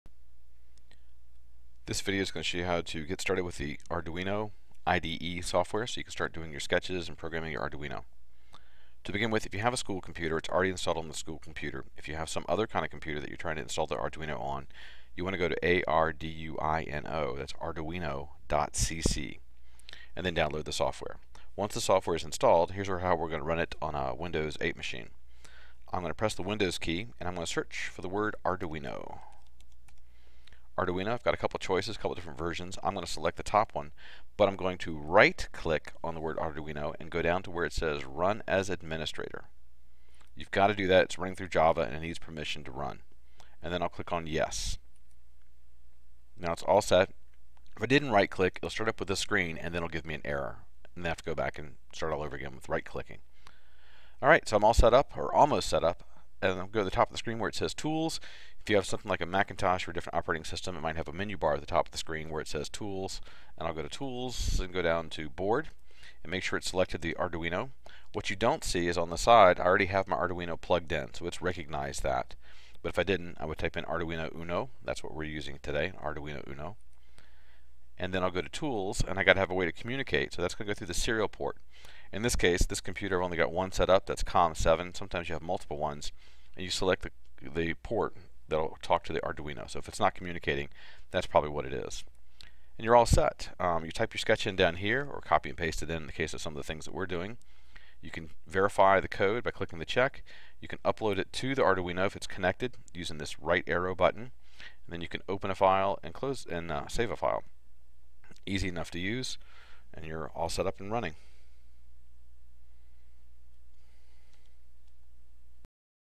Getting Started on with the Arduino IDE software on Windows 8
. Our school computers come with this software installed on it. This video shows you how to find and run it on our school computers.